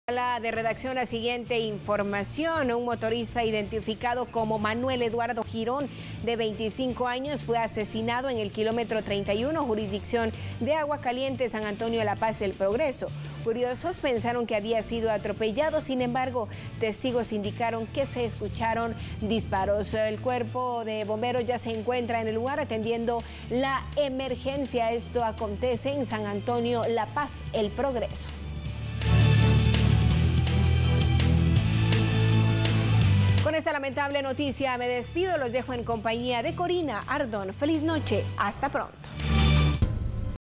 Asesinan a motorista en San Antonio La Paz, El Progreso
Bomberos Voluntarios cubrieron la emergencia.